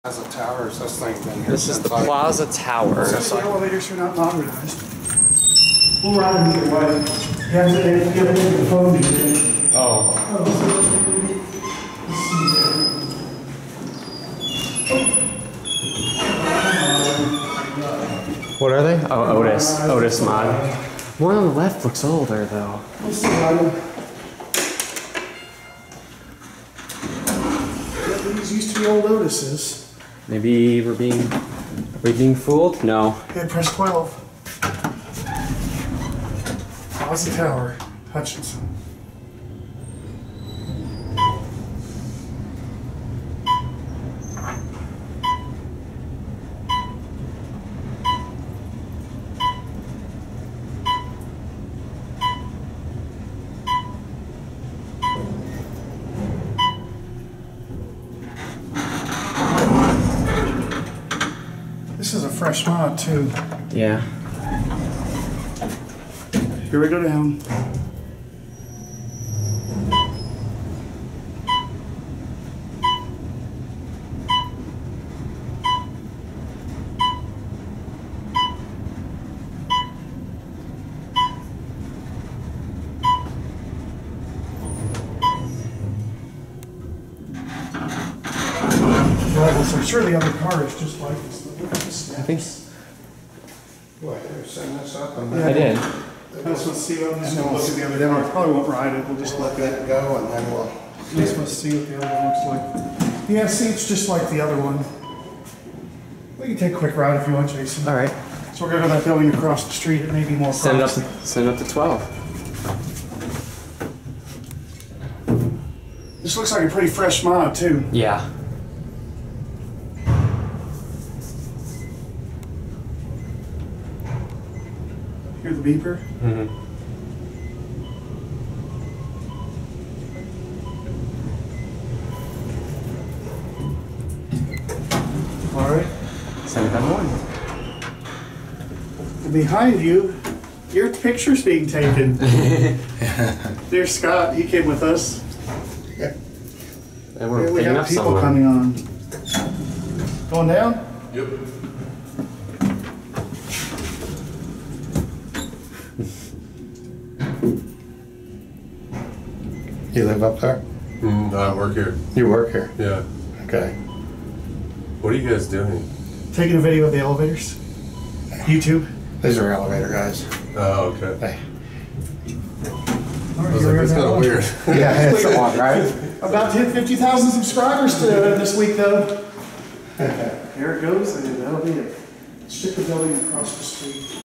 Otis Traction Elevators @ Plaza Towers - Hutchinson, KS
(5-22-18 with Dieselducy and Scott) Here are some modernized elevators in downtown Hutchinson. This is not a bad mod!